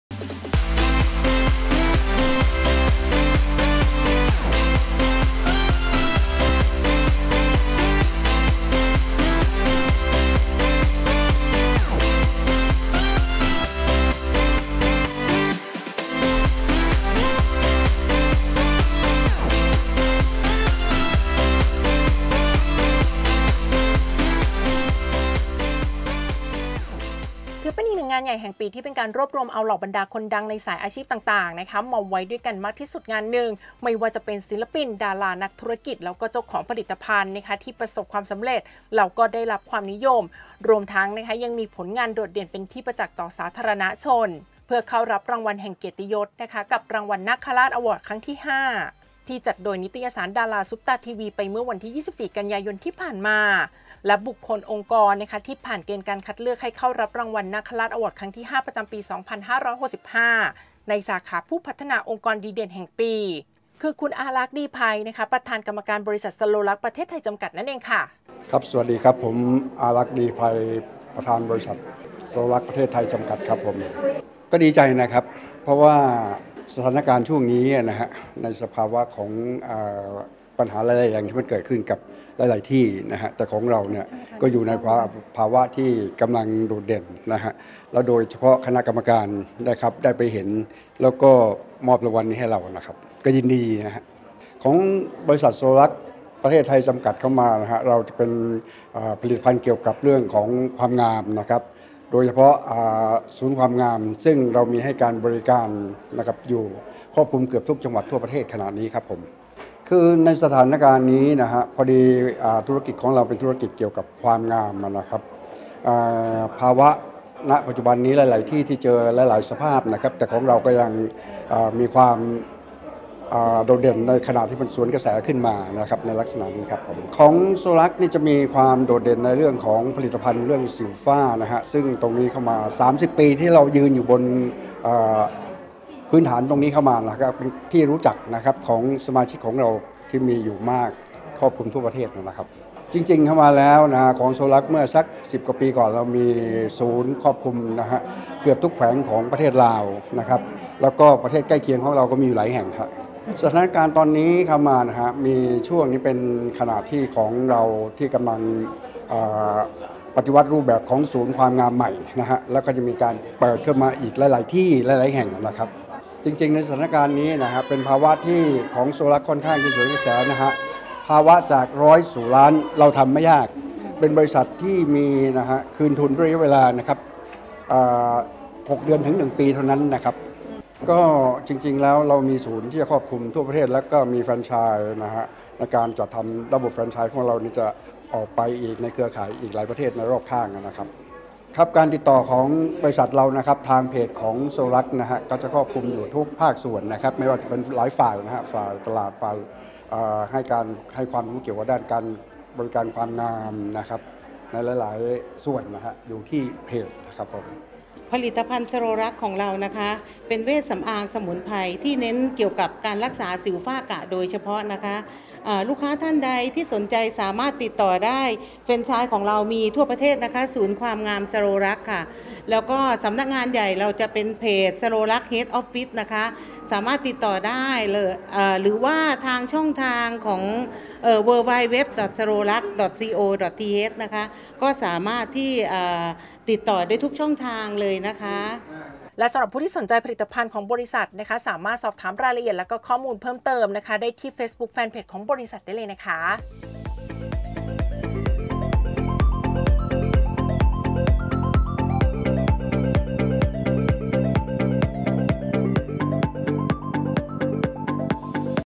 Lady New : คุณอารักษ์ ดีไพร คว้ารางวัล นาคราชอวอร์ด ครั้งที่ 5 สาขา ผู้พัฒนาองค์กรดีเด่นแห่งปี
ถือเป็นอีกหนึ่งงานใหญ่แห่งปี ที่เป็นการรวบรวมเอาเหล่าบรรดาคนดังในสายอาชีพต่างๆ มาไว้ด้วยกันมากที่สุดงานนึง ไม่ว่าจะเป็น ศิลปิน ดารา นักธุรกิจ และเจ้าของผลิตภัณฑ์ ที่ประสบความสำเร็จ ได้รับความนิยม และผลงานโดดเด่น เป็นที่ประจักษ์ต่อสาธารณะชน เพื่อเข้ารับรางวัลแห่งเกียรติยศ กับรางวัล นาคราช อวอร์ด ๕ ที่จัดโดย นิตยสาร ดาราซุปตาร์ทีวี ไปเมื่อวันที่ 24 ก.ย. ที่ผ่านมา

และบุคคล องค์กร ที่ผ่านการคัดเลือก ให้เข้ารับรางวัล นาคราชอวอร์ด ครั้งที่ 5 ประจำปี 2565 ในสาขา ผู้พัฒนาองค์กรดีเด่นแห่งปี คือ คุณอารักษ์ ดีไพร ประธานกรรมการ บริษัท สะโรรักษ์ (ประเทศไทย) จำกัด

และสำหรับผู้ที่สนใจผลิตภัณฑ์ ของบริษัท สามารถสอบถามข้อมูลเพิ่มเติมได้ที่ thailand, thailand - head office เบอร์โทรศัพท์ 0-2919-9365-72,